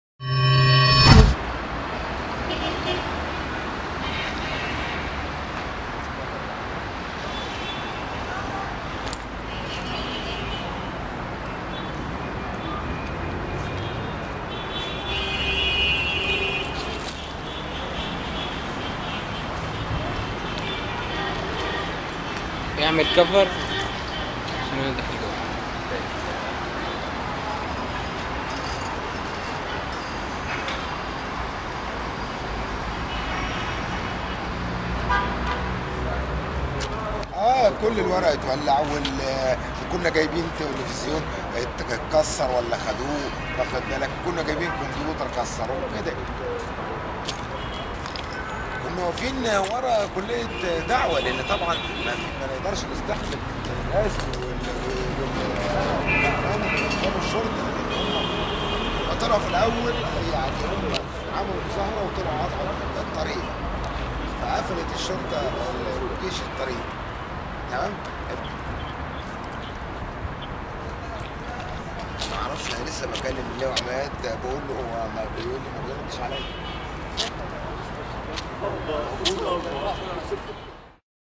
طلاب الاخوان يحرقون غرفة مدير أمن جامعه الازهر
قام عدد من  طلاب جامعة الازهر  المؤيدين لجماعة الاخوان المسلمين  بتحطيم الغرفة الخاصة  بمدير أمن الجامعة وتحطيم محتوياتها  ، بالإضافة إلي حرق عدد كبير من الاشجار .
ورصدت كاميرا "مبتدا " أثار تدمير محتويات  الغرفة ، والتى كانت تتكون من تلفزيون وجهاز كومبيوتر وعدد من متعلقات أفراد الأمن والتي تم حرقها تماما  .
كانت جامعة الأزهر قد شهدت صباح اليوم  اشتباكات حادة بين طلاب الاخوان والامن بسبب قيام المحتجين بقطع طريق النصر ،مما أدى إلى قيام قوات الامن إ بإلقاء قنابل مسيلة للدموع لإجبار المحتجين  على العودة إلي داخل الحرم الجامعي .

تصوير: نادر أمين  - مصطفي شاكر